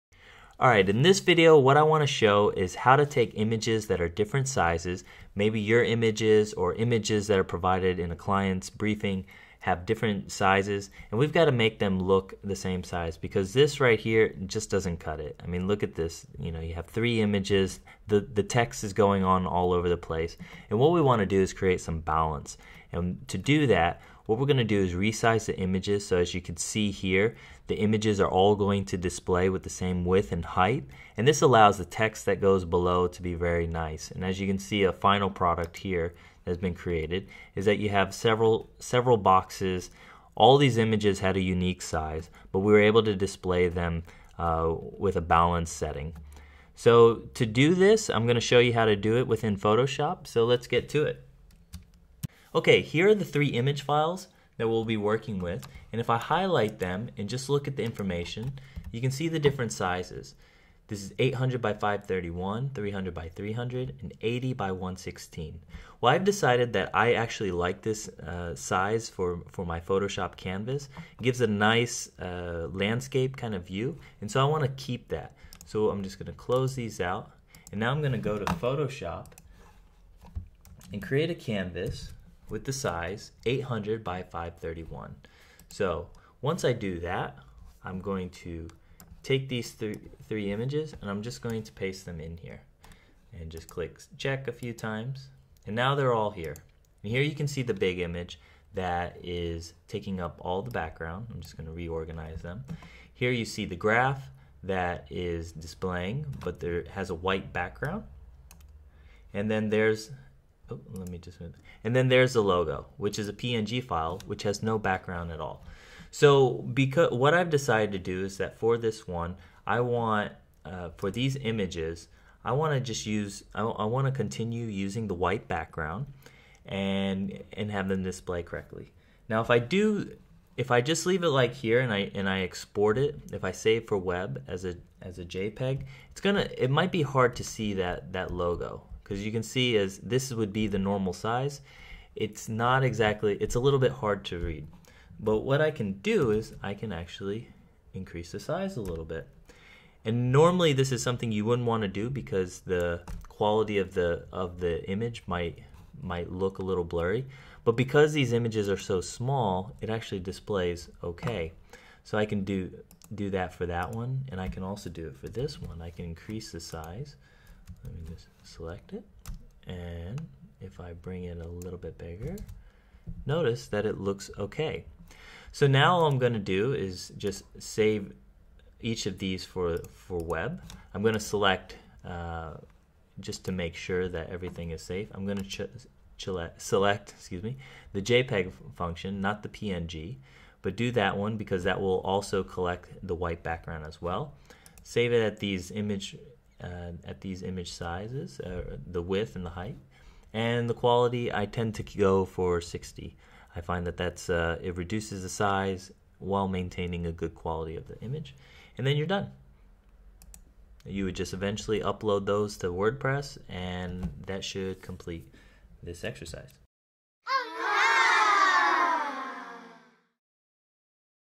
Getting various image sizes to display with the same size
When creating a website, it's essential to use a nice balance of text and images.  Sometimes images that we must use; however, are not always the same size.  Unless we make some manual changes, the images will not look nice and so this is a tutorial showing ways to manipulate photos and get a final product that feels balanced